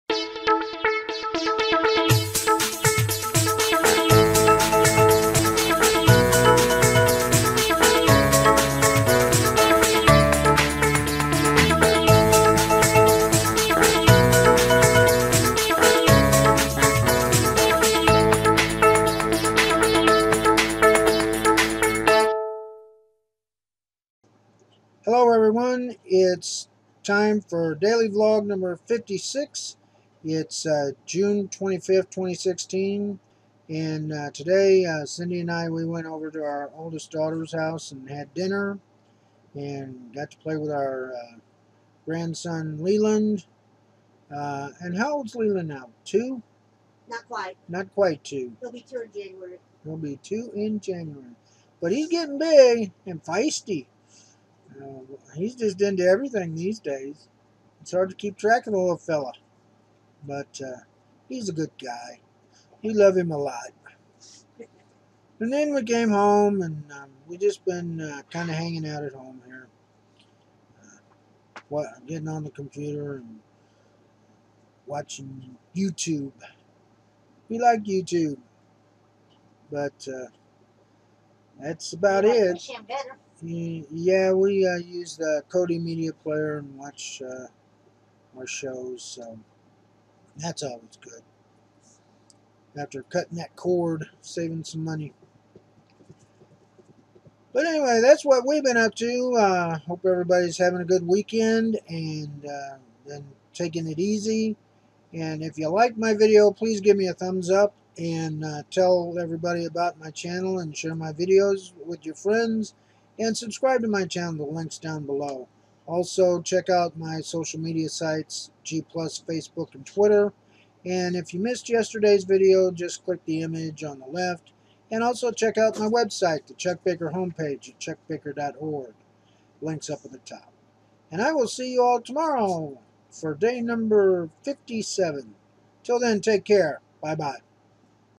Daily Vlog 56 June 25 2016
Here is my Daily Vlog 56
Dinner at our oldest daughters house